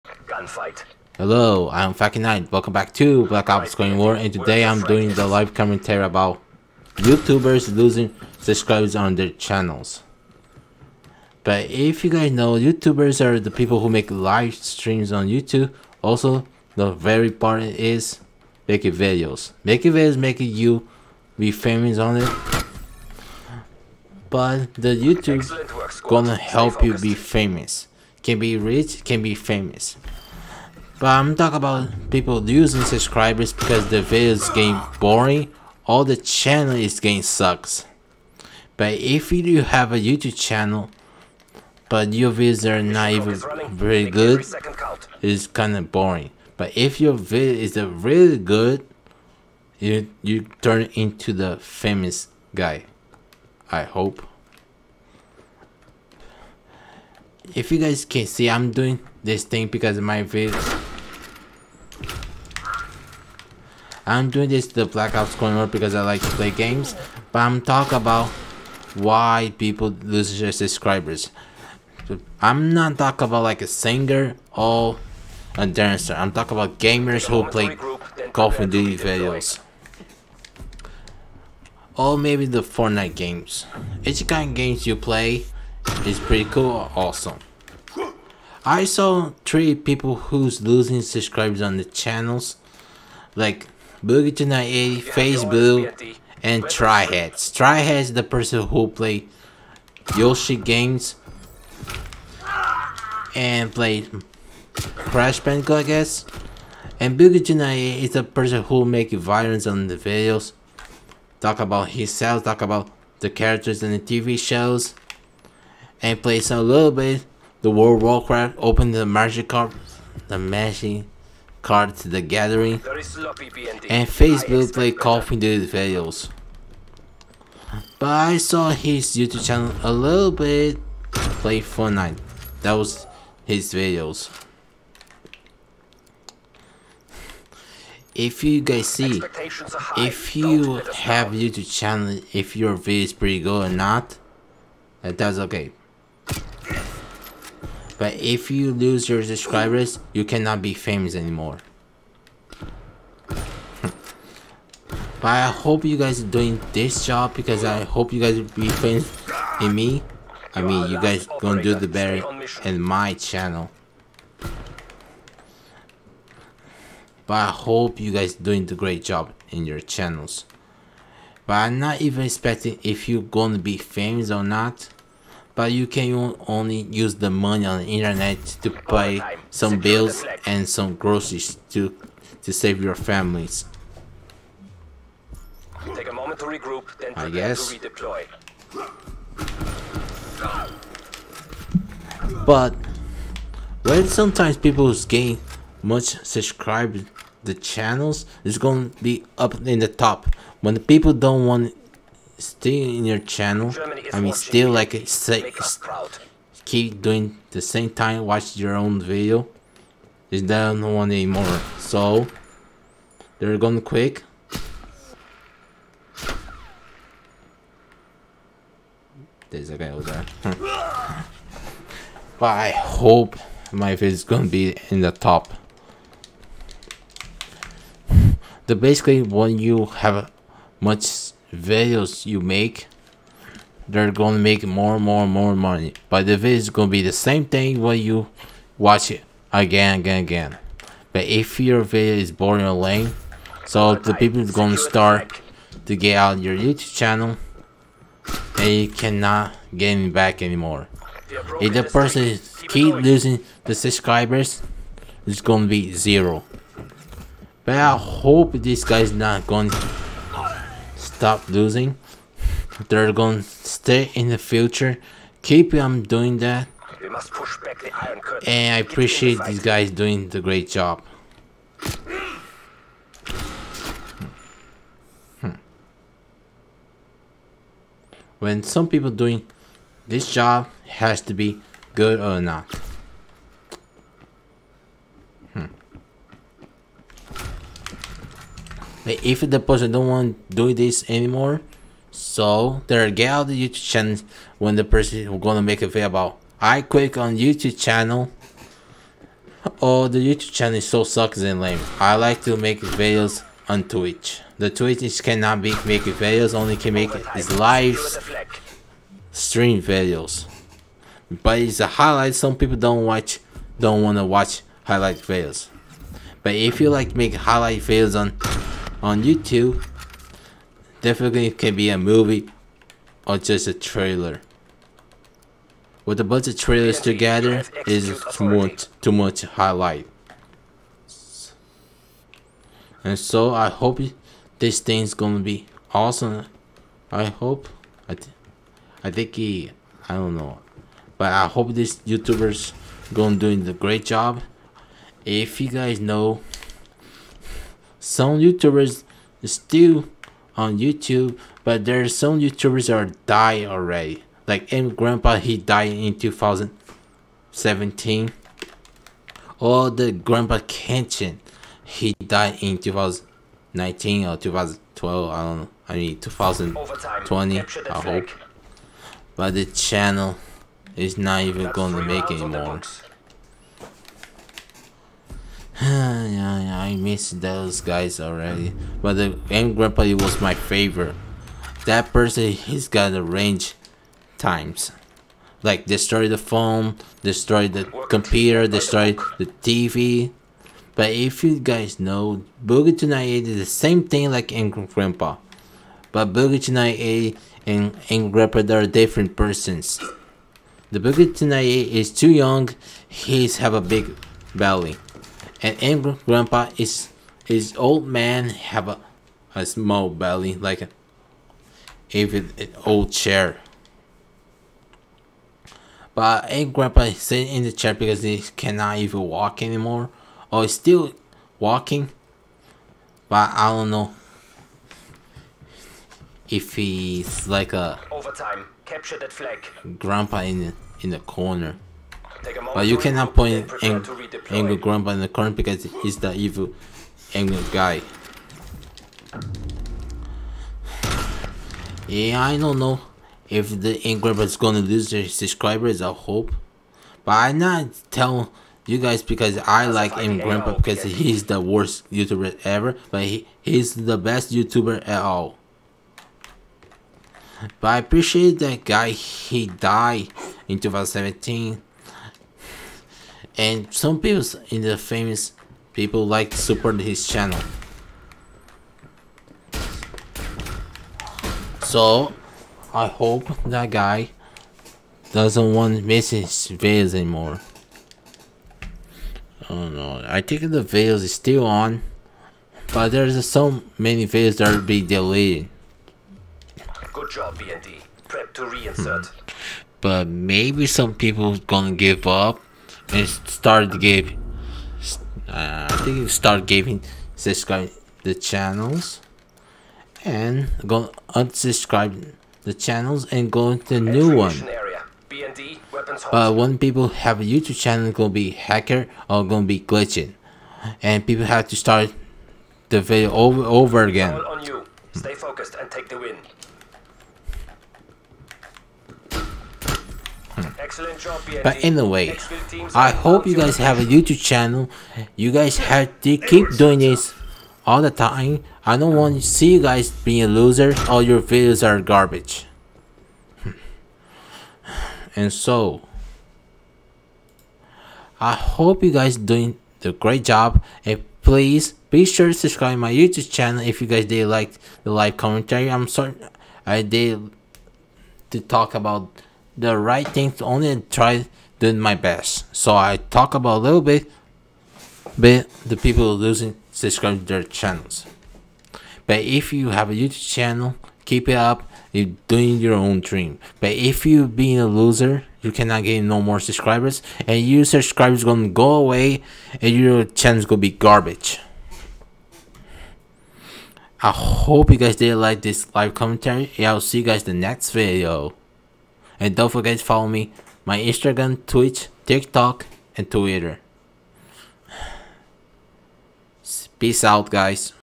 Fat Kingnight | live commentary / youtubers losing subscribers them channels?
if you guys know the famous  youtubers have a all of lot  subscribers them channels! and now started to losing many subscribers on them channels?.